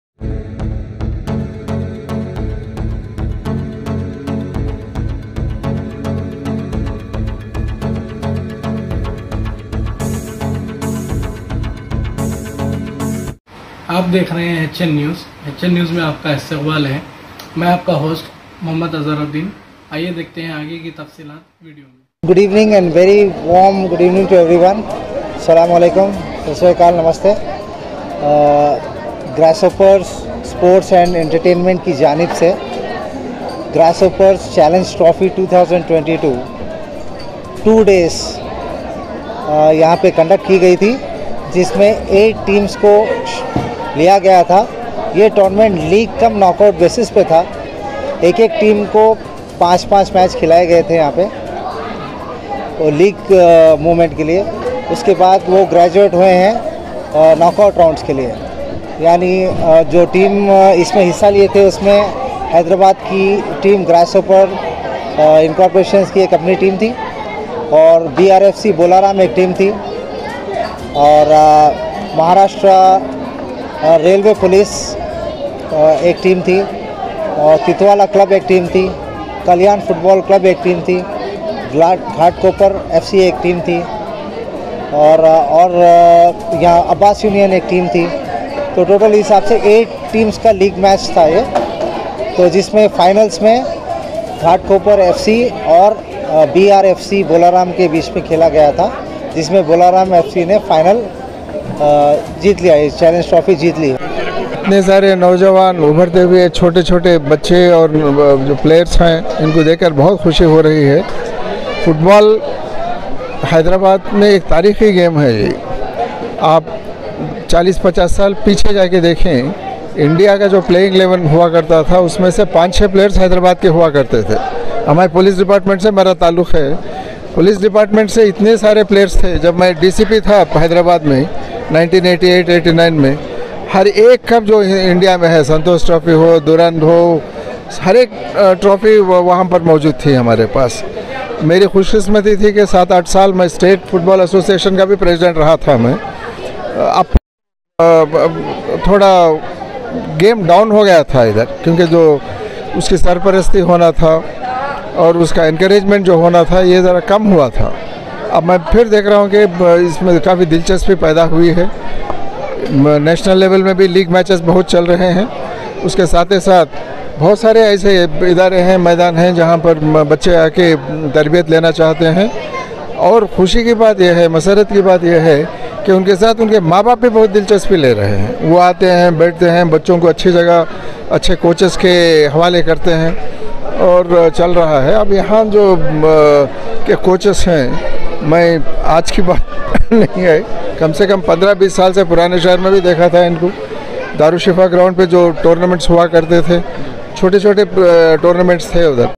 Grasshopper's sports & Entertainment incorporations Conducted Challenge Football Trophy 2022
on 15 and 16th February 2022 Grasshopper's sports & Entertainment incorporations Conducted Grasshopper's TV Challenge Football Trophy 2022 Hotfut turfgrounds at begumpet powered by Prakruti Avenues Pvt Ltd where in .teams like Mumbai Railway Police boys , Kalyan football club, Ghatkoper football club,Thane FC ,BRFC bolaram ,Team Grasshopper's ,Abbas Union ,Sahara Mumbai FC , participated this Tournament was on league cum knockout basis ..where in BRFC BOLARAM Clinched the Title ..with ghatkopar FC in finals .Mr agha Muzaffar Uddin director Grasshopper's sports & Entertainment organised this Tournament . Chief Guest Mr A.K Khan for mer commission of police gave away the tropies to the Winners and 1st & 2nd Runner ups..first runners up was ghatkoper FC from Mumbai and third place was Sahara Mumbai FC ..in event former natinal and International football players were spotted to grace the occasion and encourage young talents .